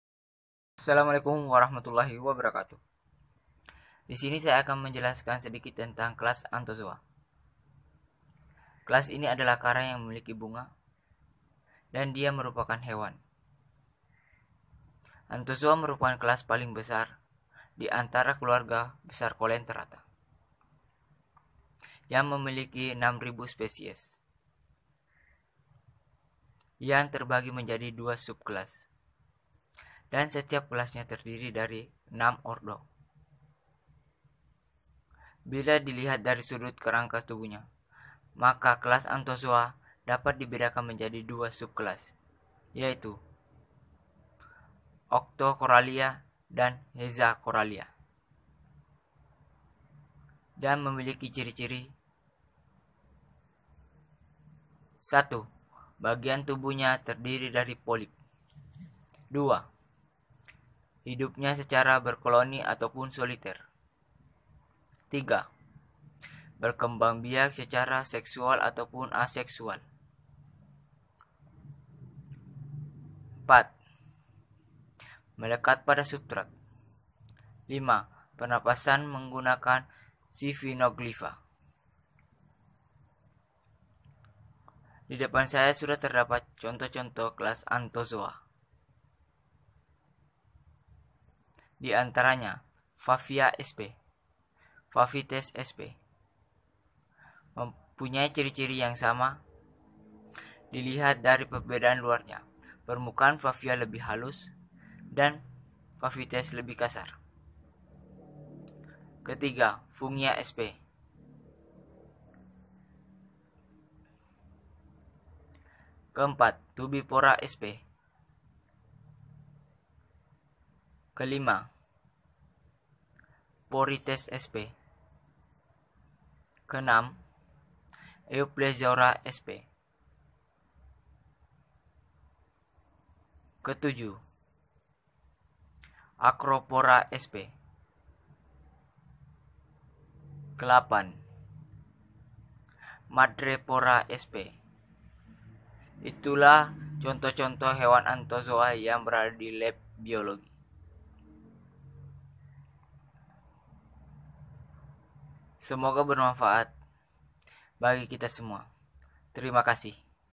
Avertebrata kelas anthozoa (penjelasan ringkas)
maaf ya teman didalam video ada yg tertukar antara 4.tubipora sp.  dan 8  madrepora sp.

semoga bermanfaat